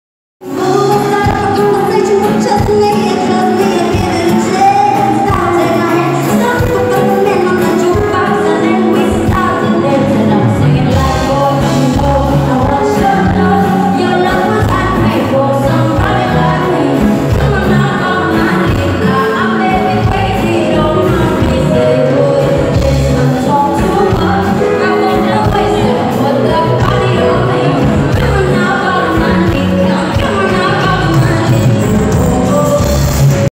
Manila X 2017 ft KZ Tandingan
Sorry sabog ung audio.. katabi kc kme ng speaker..
Umulan kc kaya pinapasok kme sa my both side ng stage..
Hinaan nyo nlng po ung speaker nyo heheheh..
Pasensya na po ulit..